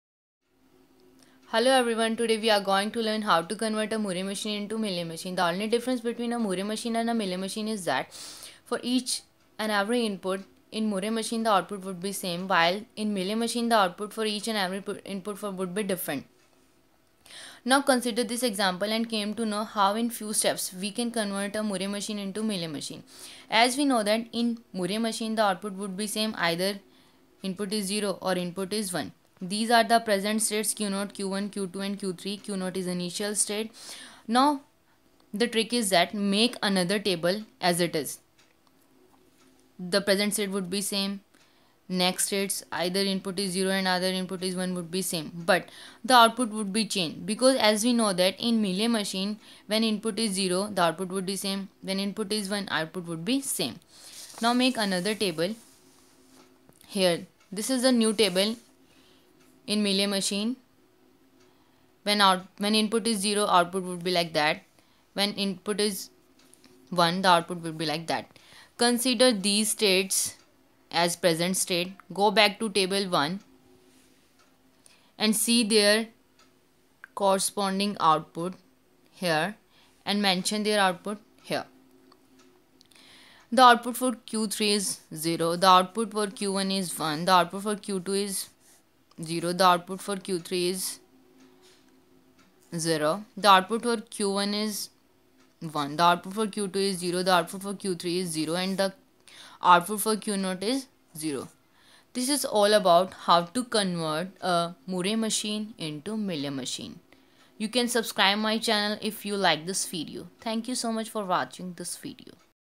How to convert Moore machine to Mealy machine (Theory of Computation)
Tutorial ( theory of computation )